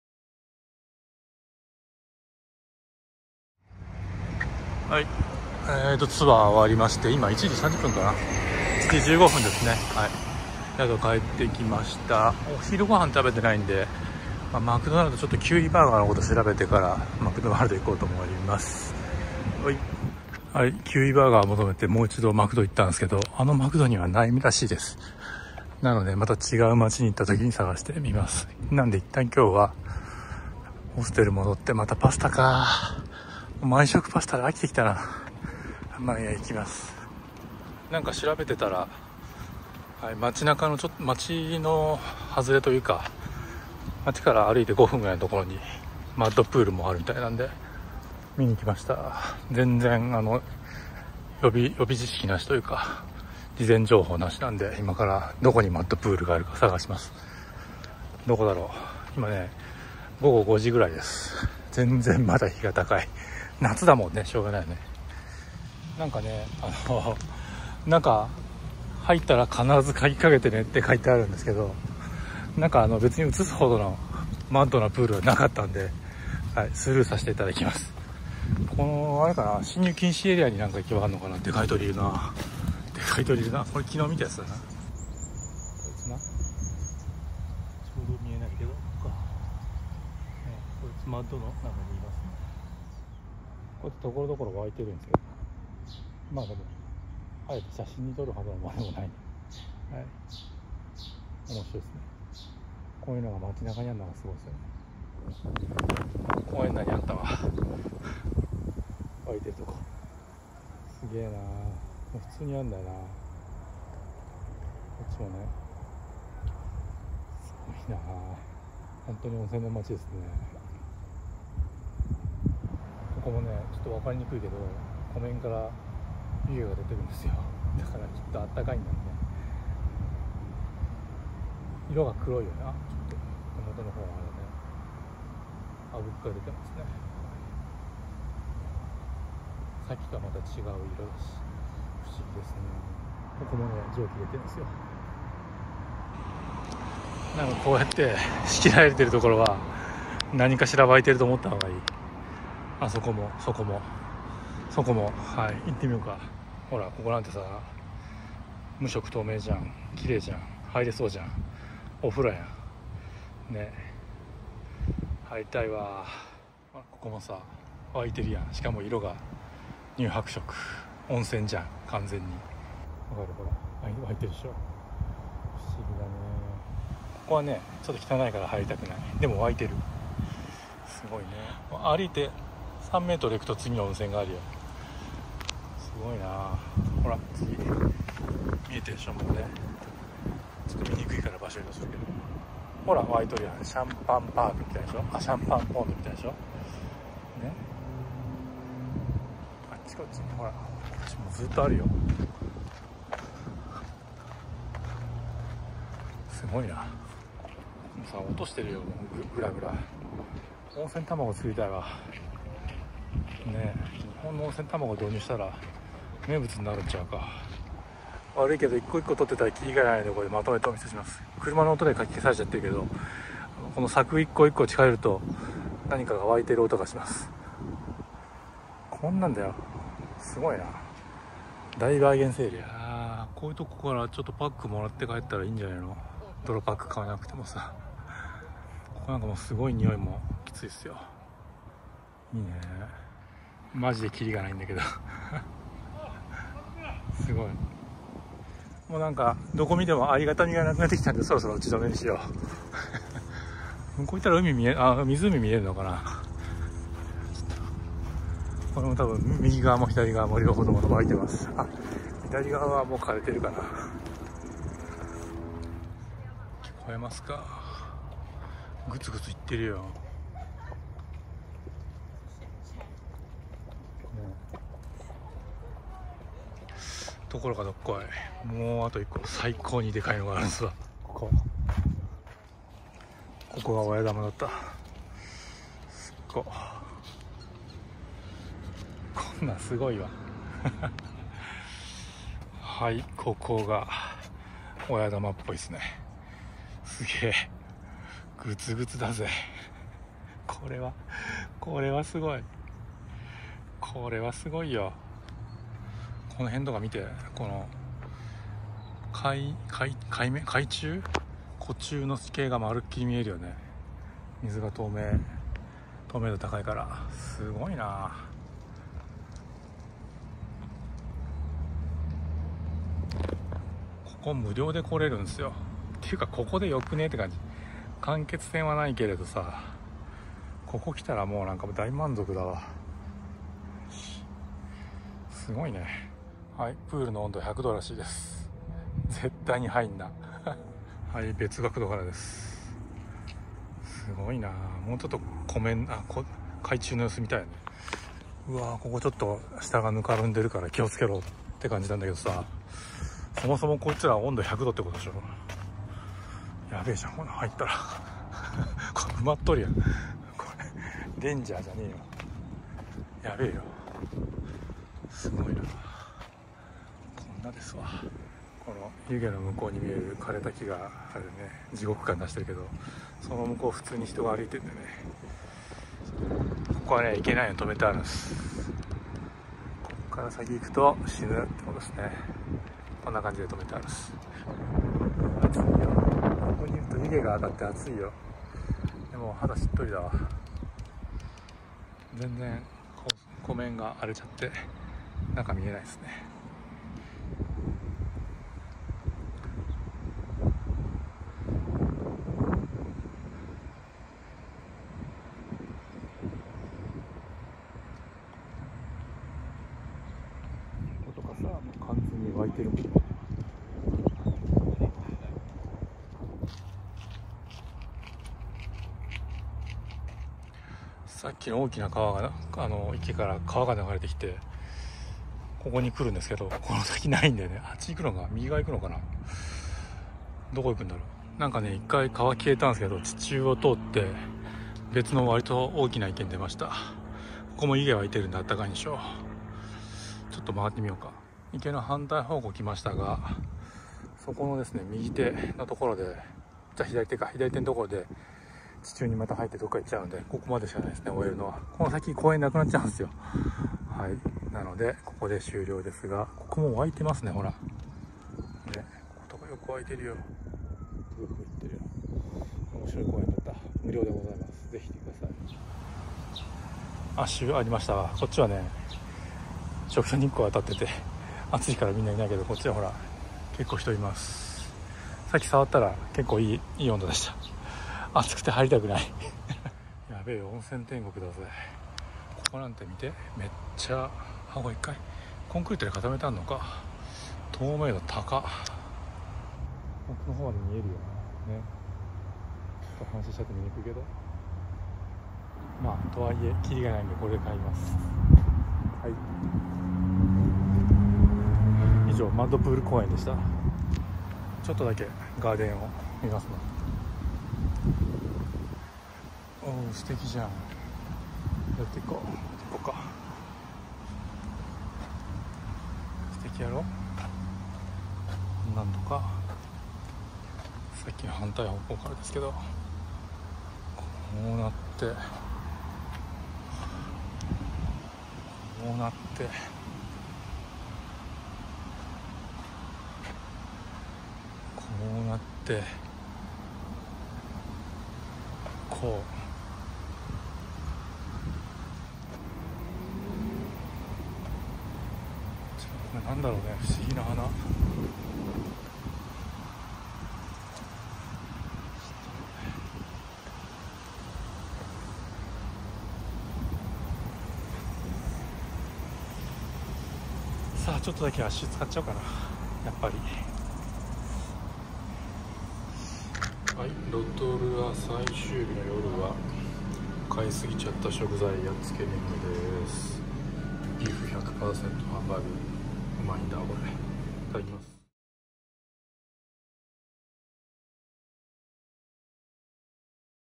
2024 ニュージーランドp12 クイラウ公園 ロトルア
ロトルア市民に親しまれているクイラウ公園を訪れます。
ここは無料でロトルアの地熱を楽しめます。